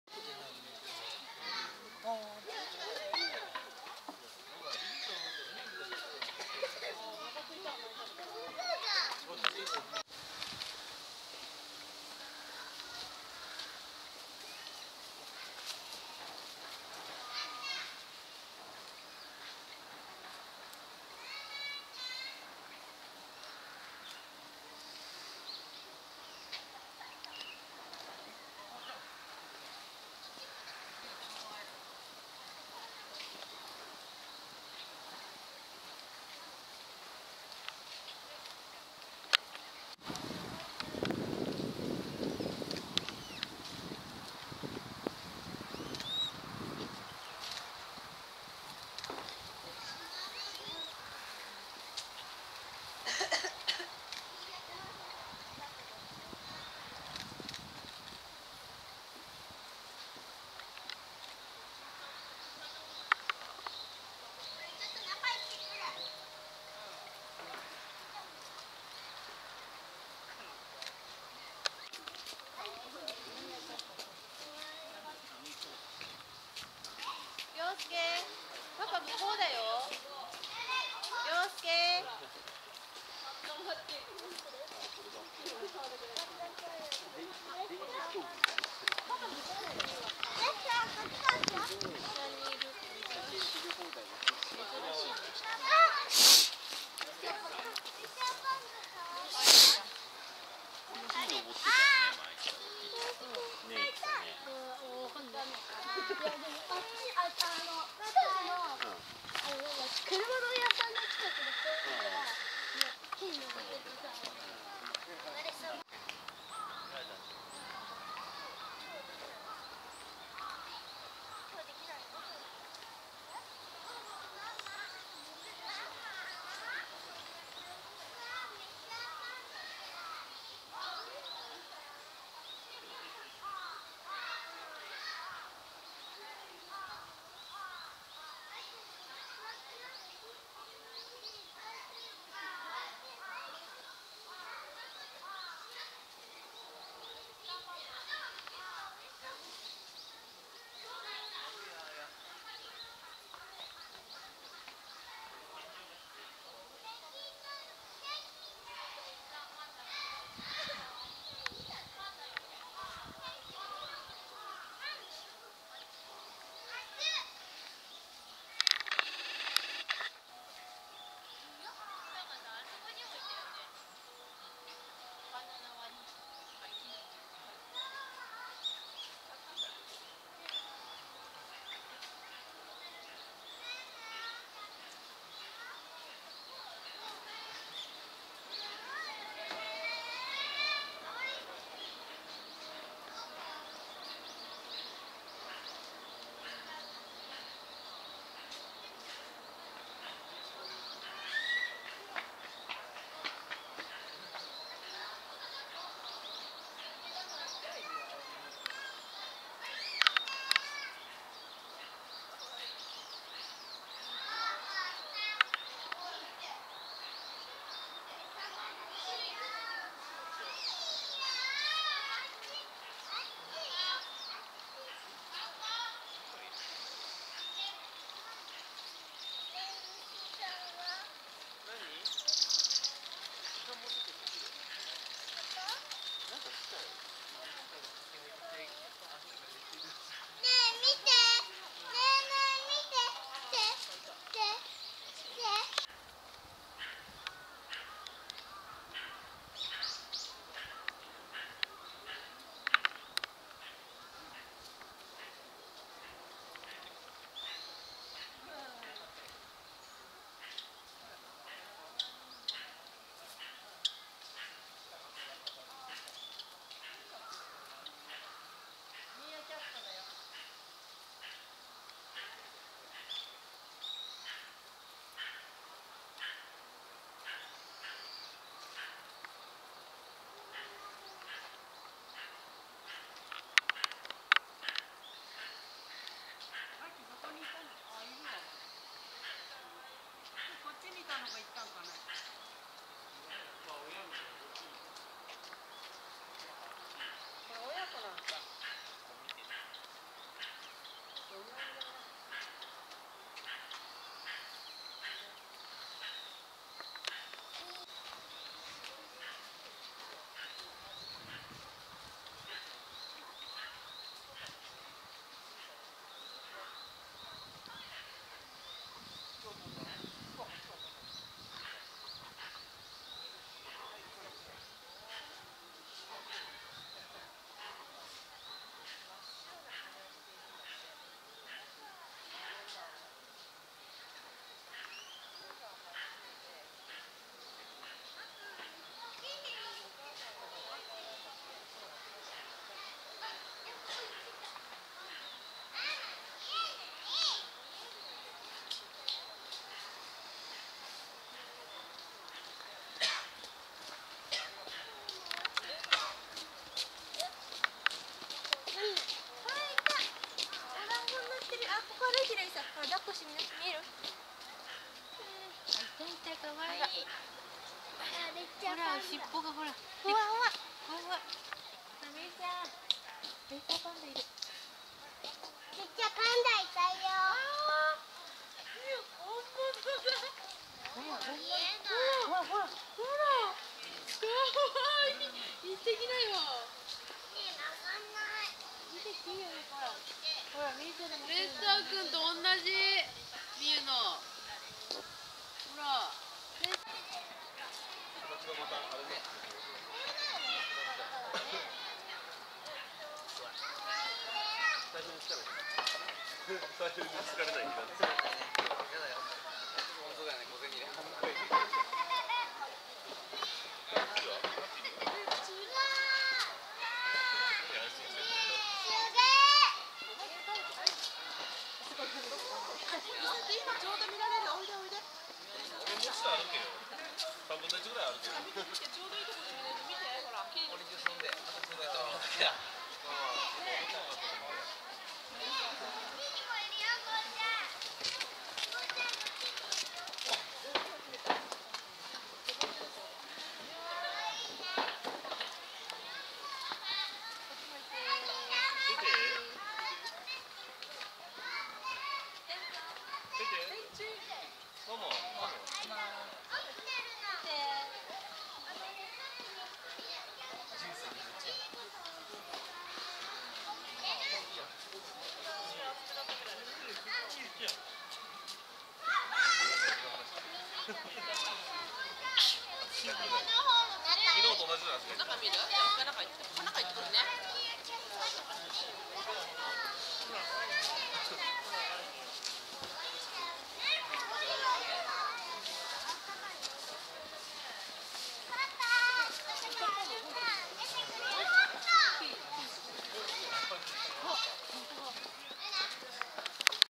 2013.12.23 埼玉県こども動物自然公園 レッサーパンダ リリィ Saitama children's zoo Red panda Lily
1：30くらいから少し映像の乱れがありますが、ご了承ください。
リリィと一緒に歩いているのはリリィの息子、7月10日生まれの双子の兄キャラです。

There is a disturbance of the video from around 1:30, please understand.
A son (cara) who was born in July 10 as for walking the back of Lily.